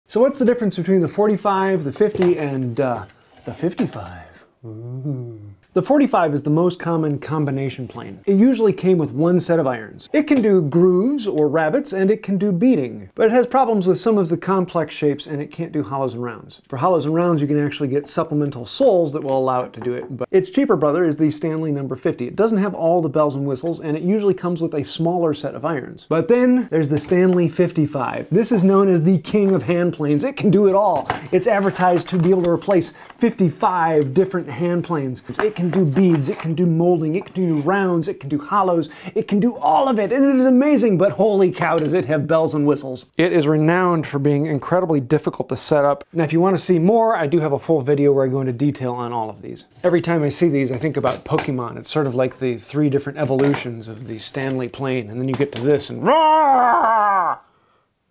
What Makes a 55 better then a 45 or 50?
The Stanley 45, Stanley 55 and Stanley 50 are all combination planes that can do a bunch of different tasks.

Andrew Wilson
Brian Suker
Kenny-Anjanette Horn
Christopher Brown
Alex Adams
Ian McElcheran
Russell Gough